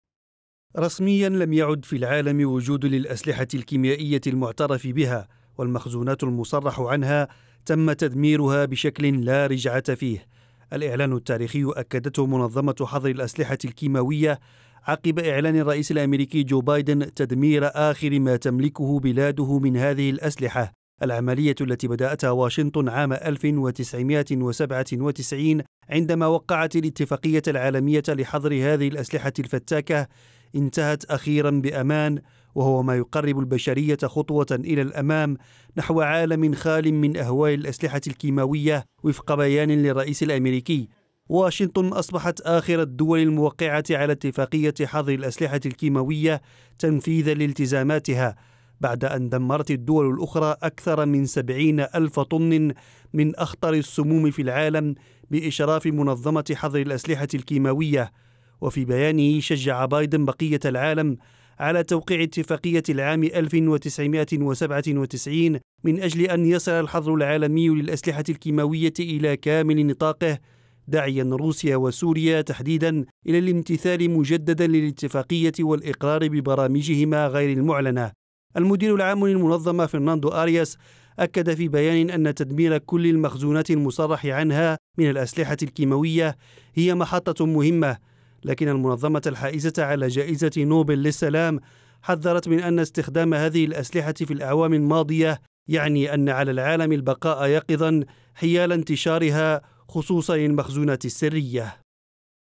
الرئيس الأميركي يعلن أن بلاده دمرت جميع مخزوناتها من الأسلحة الكيماوية
أعلن الرئيس الأميركي جو بايدن، أن بلاده دمرت جميع أسلحتها الكيماوية، لتستكمل عملية بدأتها أواخر تسعينيات القرن الماضي.
وأصبحت الولايات المتحدة بذلك، آخر الدول الموقّعة على اتفاقية حظر الأسلحة الكيماوية تنفيذا لالتزاماتها.